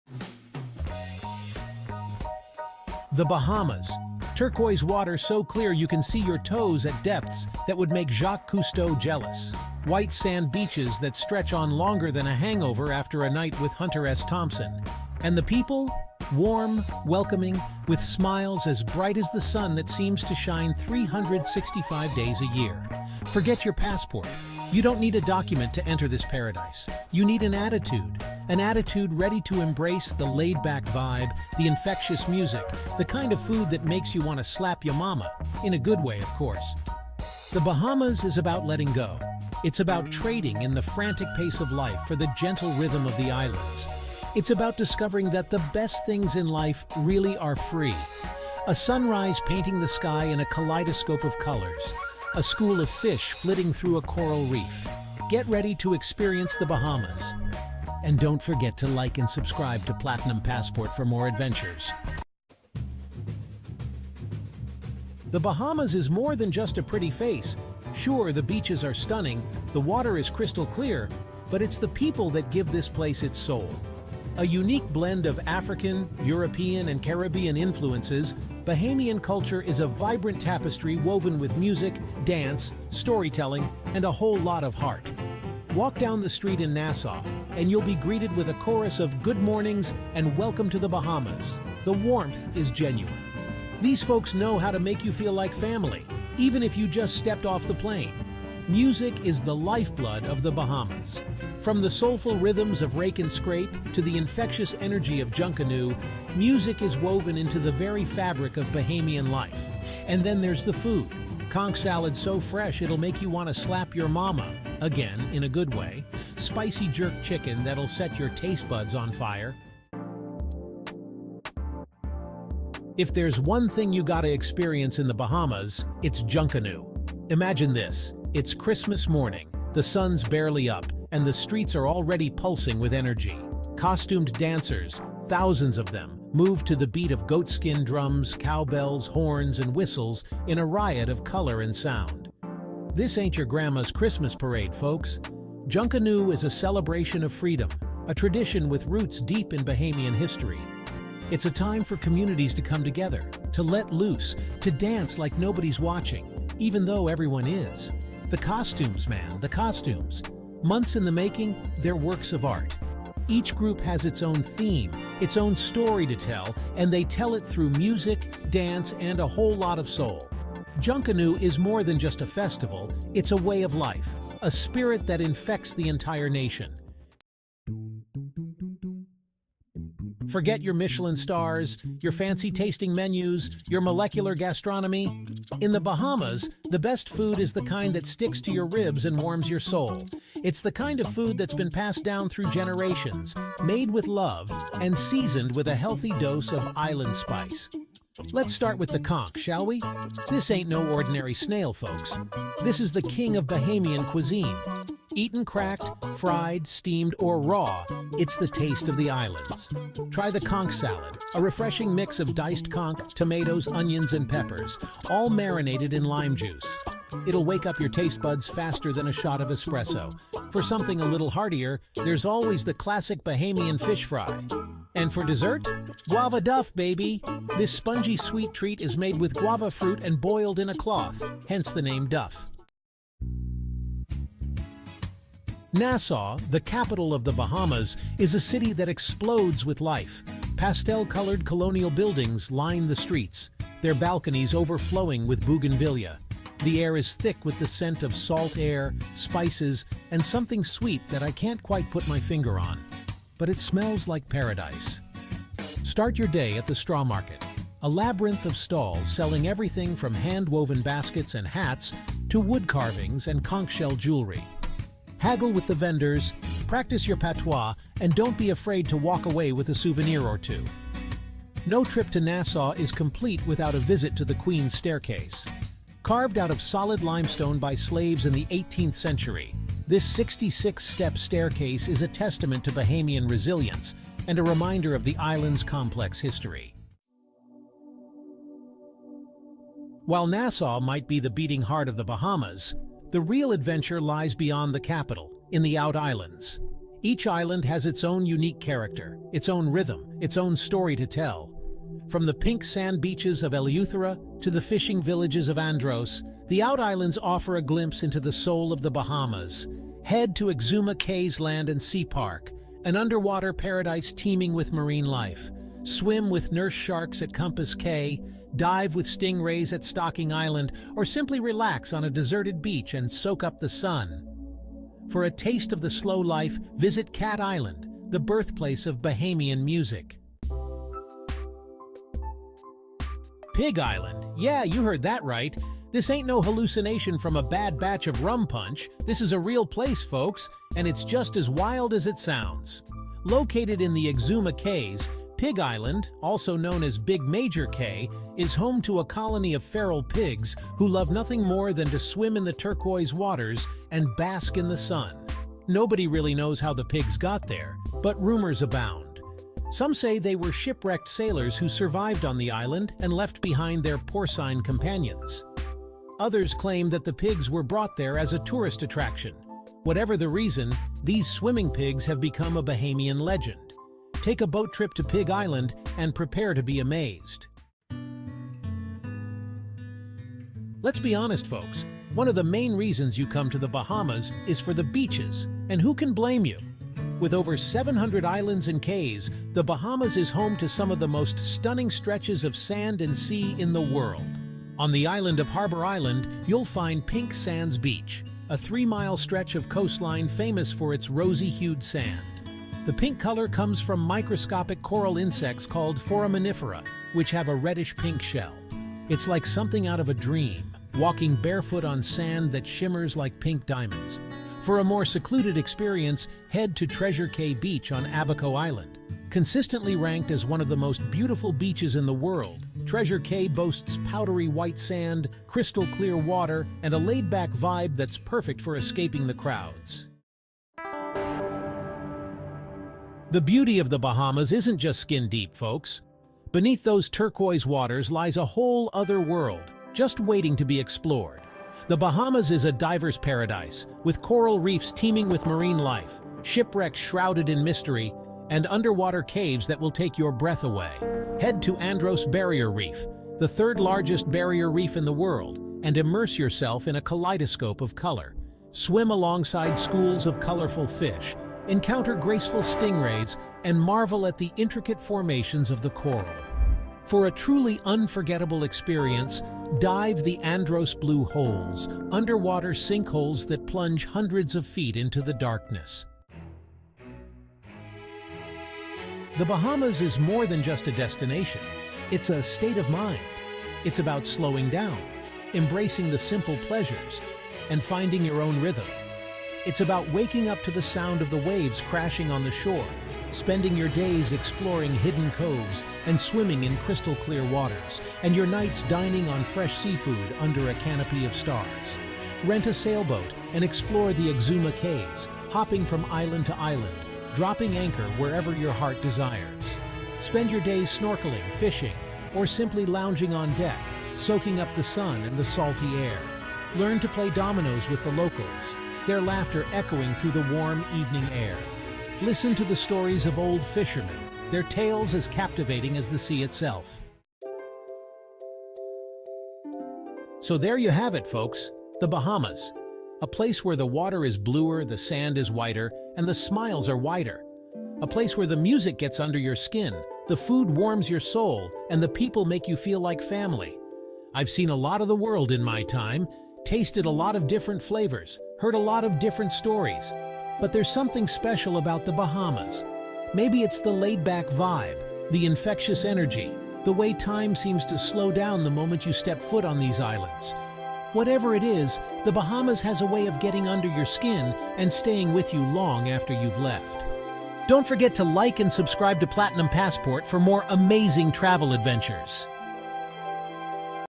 "Discover Paradise: Your Ultimate Guide to Traveling to the Bahamas!"
🌴 Welcome to paradise! In this video, we take you on an unforgettable journey to the Bahamas. 🌊 From pristine beaches and crystal-clear waters to vibrant culture and exciting adventures, the Bahamas has it all. Join us as we explore the best spots, share travel tips, and reveal hidden gems to make your trip extraordinary.